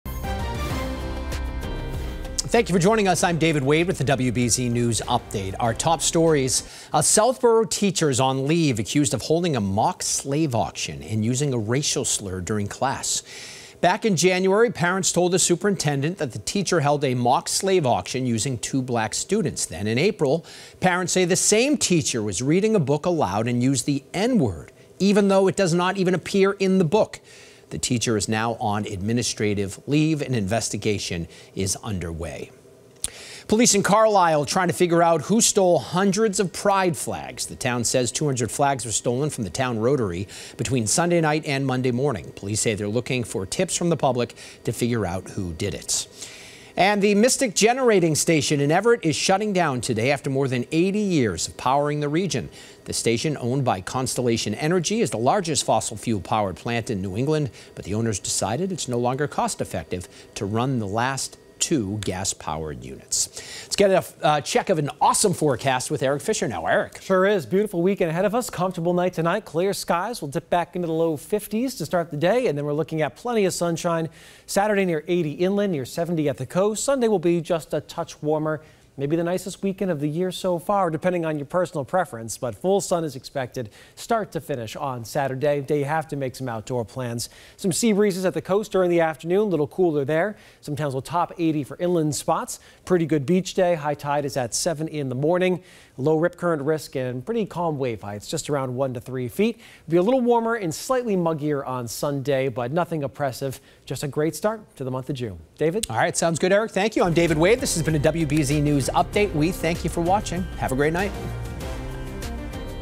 Pride flags stolen in Carlisle and more top stories
WBZ-TV's David Wade and Eric Fisher have the latest news and weather.